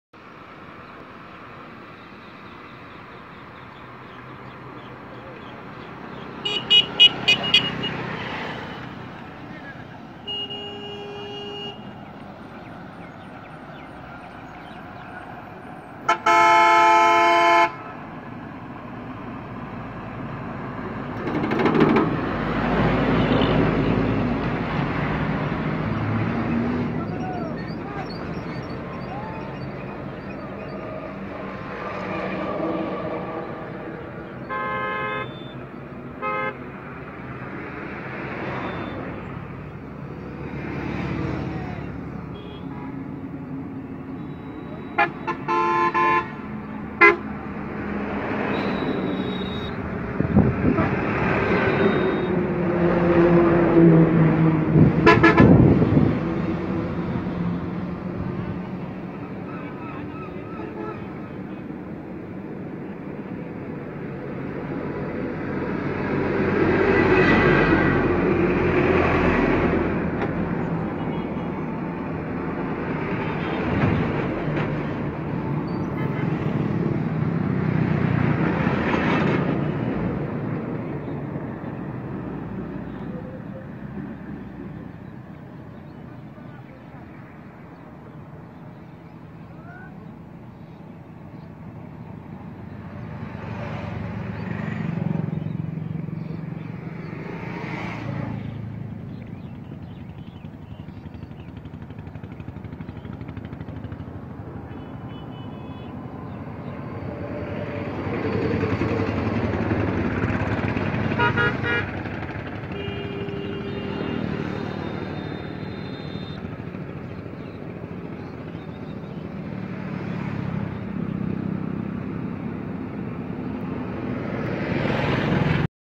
Wonderful Forest Flower Beside the Highway - Beautiful Wild Flowers.
Beautiful wild flowers - Wonderful forest flower beside the highway - If you don't see it with your eyes, you wouldn't believe that wild flowers can be so beautiful.